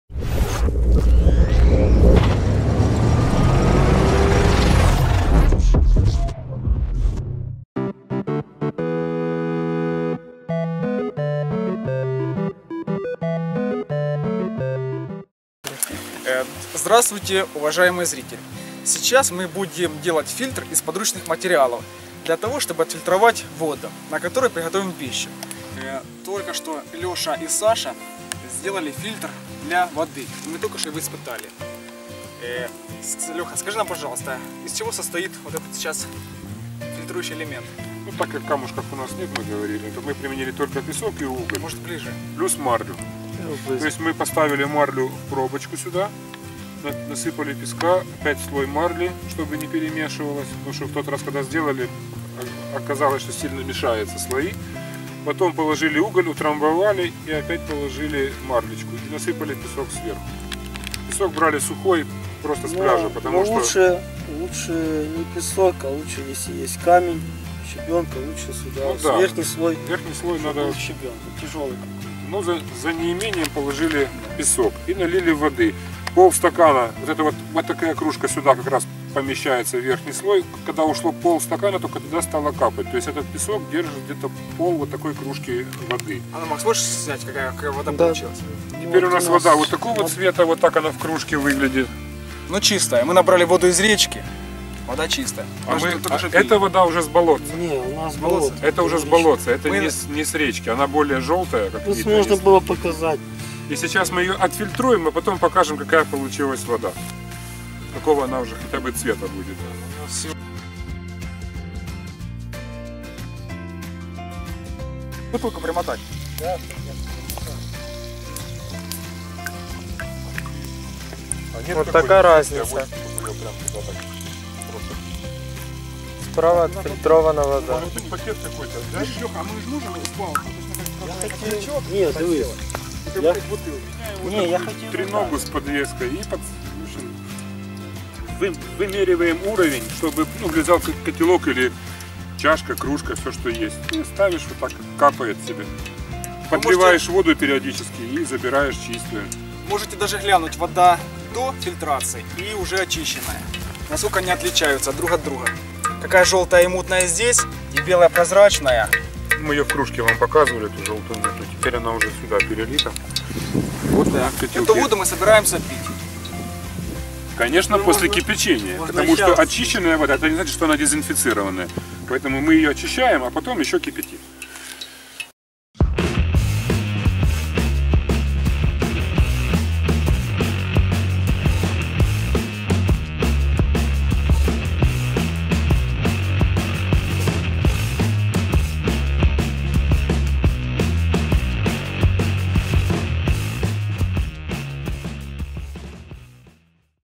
Как сделать фильтр для воды в походных условиях
В этом видео мы расскажем, как сделать фильтр для очисnки воды, если у вас нет с собой отфильтрованной воды для приготовления пищи.

Если вы забыли взять с собой воду или если у вас не было возможности ее с собой взять, можно взять воду из речки или из другого водоема отстоянной воды или проточной реки.
После этого ее нужно механически очистить, после чего прокипятить, чтобы убить все бактерии.

➨Для изготовления механического фильтра вам понадобится:
1. Марля медицинская.
2. Уголь толченный из костра.
3. Песок речной.

➨Метод изготовления:
Выложите слоями: песок, уголь, песок/мелкая галька, каждый слой разделите между собой марлей, чтобы они не смешивались между собой.